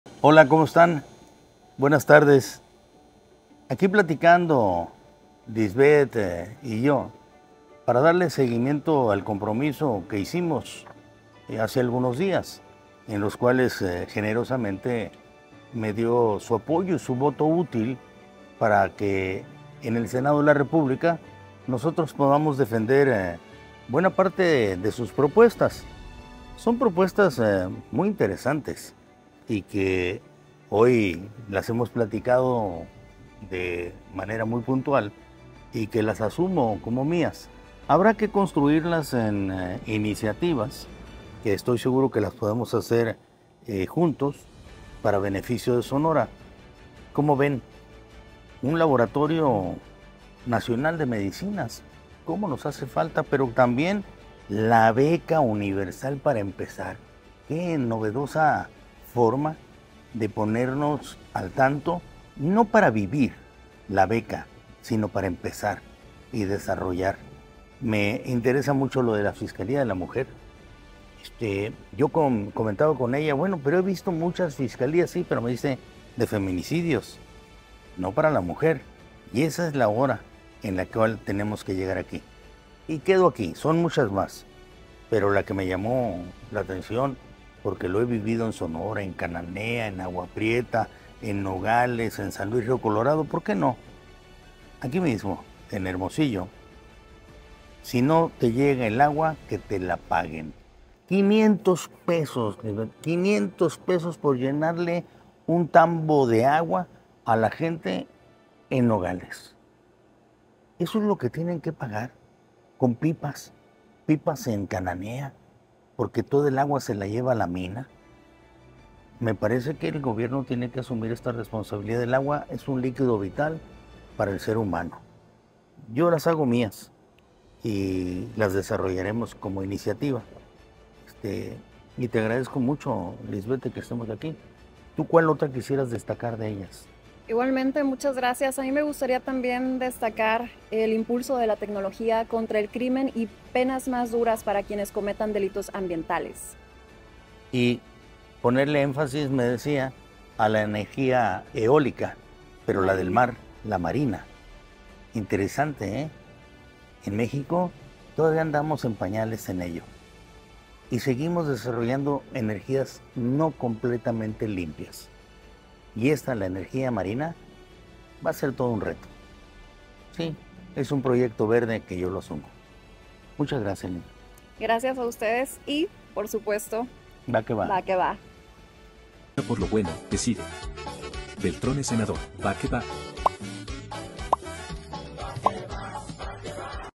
Asumo las propuestas del PVEM en Sonora rumbo al Senado de la República Vota
Junto a Lizbeth Gutiérrez, @lapoliverde, asumo las propuestas en las que trabajaremos desde el Senado en materia de salud, educación, una fiscalía enfocada en feminicidios, acceso al agua, generación de energía eólica marina. Este 2 de junio haz tu VotoÚtil! Vota